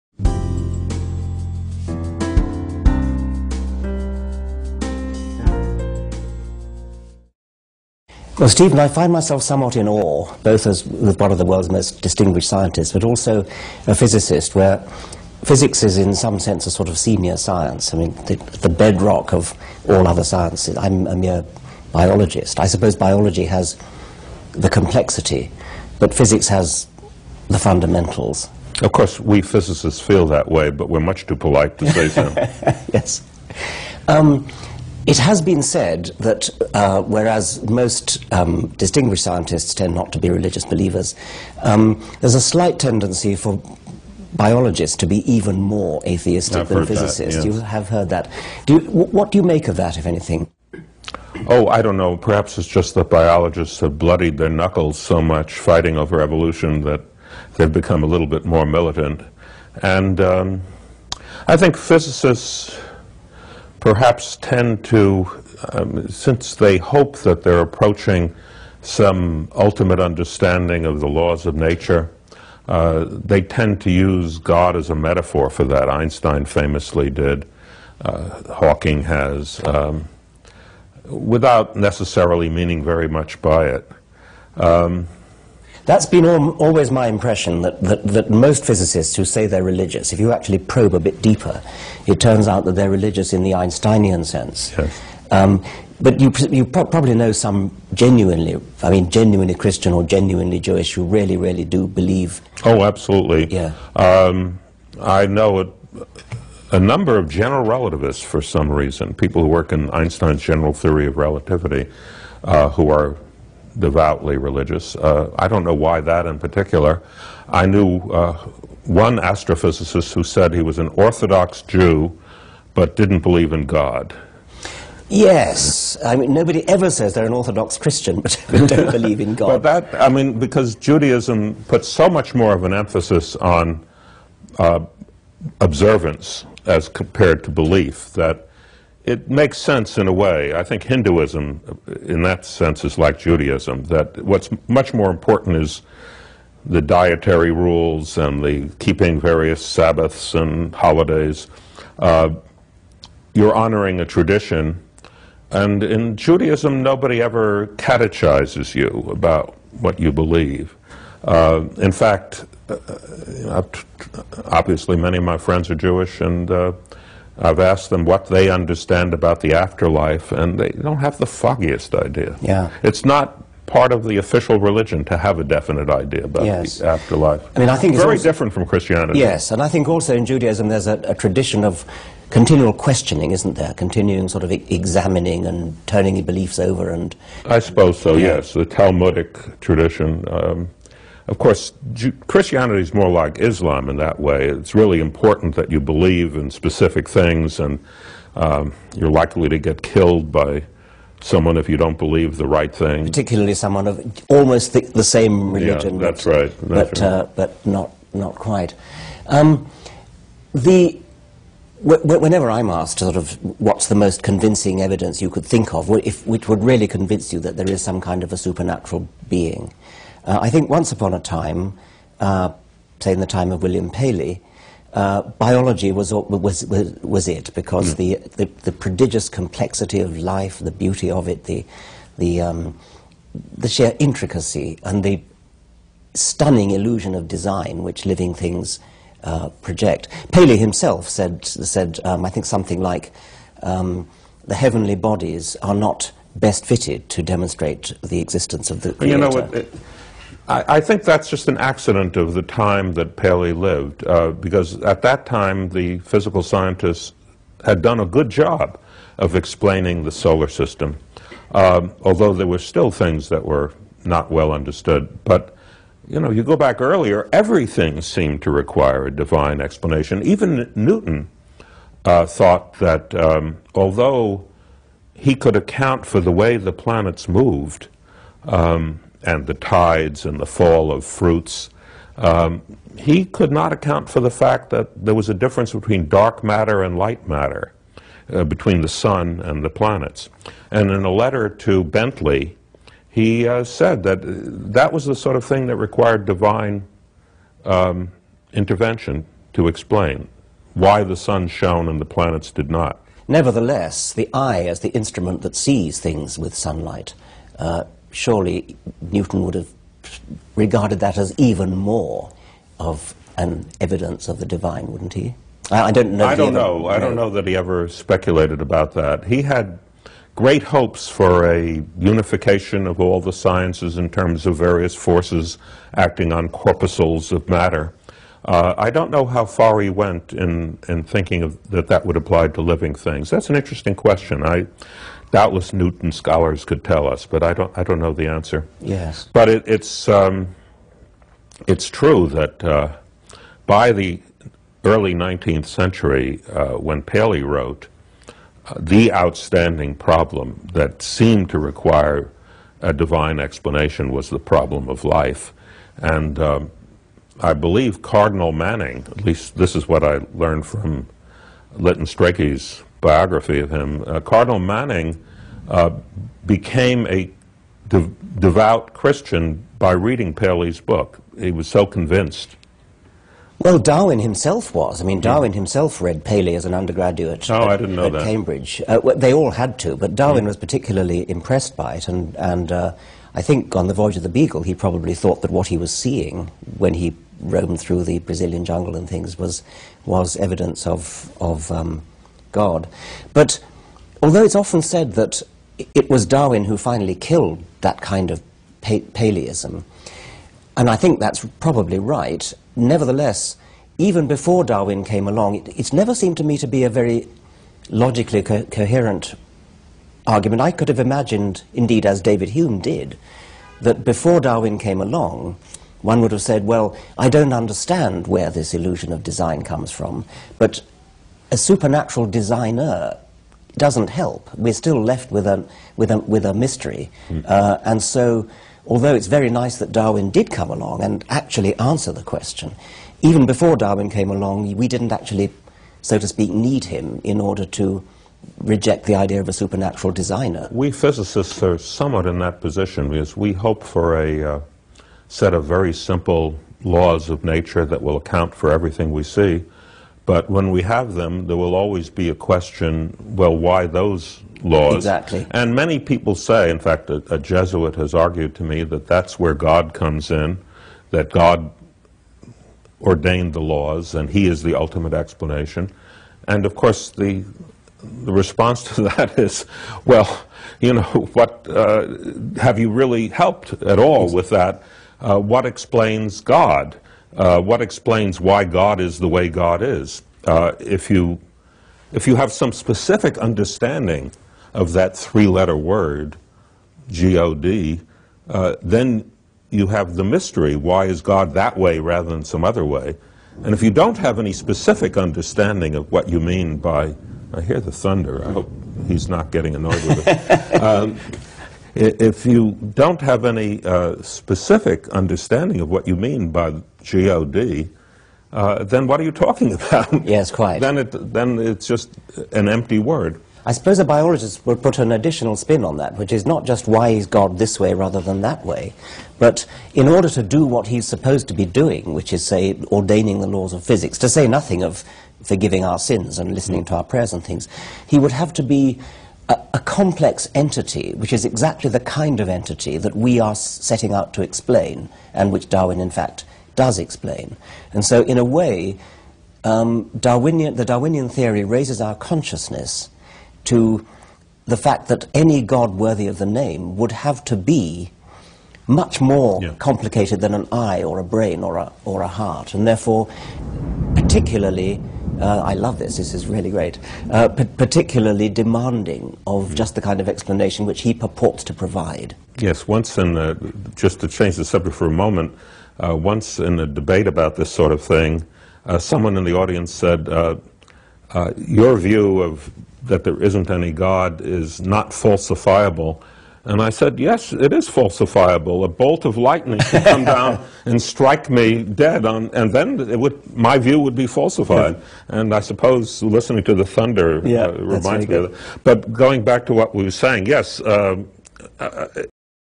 Discussion between Richard Dawkins and Steven Weinberg Part 1
DURING HIS U.S. TOUR in 2008, Biologist and bestselling author Richard Dawkins met with some of the world's leading scientists to discuss topics such as Quantum Physics, Biology, Evolutionary Psychology, Science education, religion, atheism and more. This video brings you the fascinating unedited discussions between Richard Dawkins and Nobel Prize-winning Physicist Steven Weinberg, Physicist Lawrence Krauss, Biologist and blogger PZ Myers, and Evolutionary Psychologist David Buss.

From a public conversation at Stanford University to private conversations in Austin and Minneapolis, this collection offers a rare and inspirational opportunity to observe some of today's top scientists as they discuss some of the big issues without interruption.

Credits:
Produced by Josh Timonen and Maureen Norton
Edited by Josh Timonen
Camera by Josh Timonen